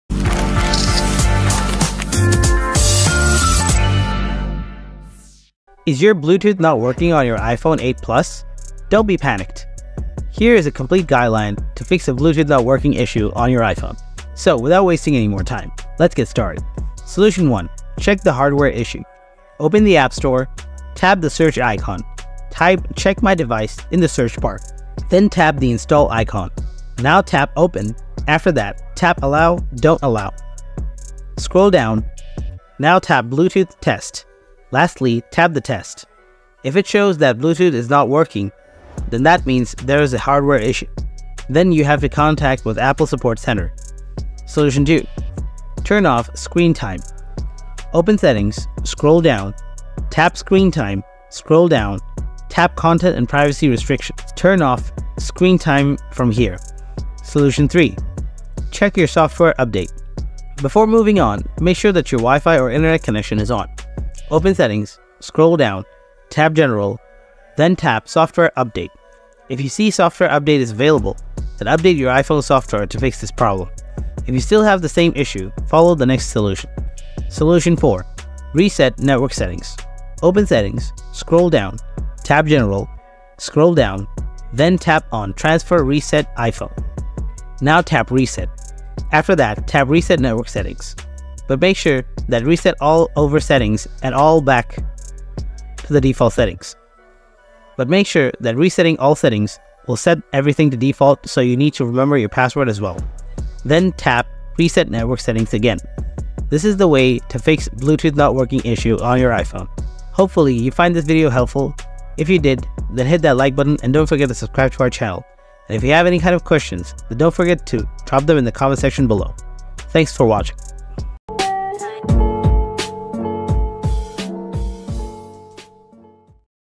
How to Fix iPhone 8 Plus Bluetooth Not Working Issue
Experiencing issues with the Bluetooth on your iPhone 8 Plus? Is your Bluetooth not working on the iPhone 8 Plus? No worries, you may think "Why is my Bluetooth not working?" or "Why did my Bluetooth suddenly stop working on my iPhone 8 Plus?" I've come up with a solution video on how to fix Bluetooth not working.

In this video, I will demonstrate the necessary steps to address Bluetooth malfunctions on your iPhone 8 Plus. By carefully watching the video and diligently following the provided method, you will be able to effectively resolve any Bluetooth-related problems on your iPhone 8 Plus.

00:01- Video intro
00:16- Solution 1: Check hardware
00:43- Solution 2: Turn off screen time
00:56- Solution 3: Check software updates
01:16- Solution 4: Reset network settings
01:45- Solution ending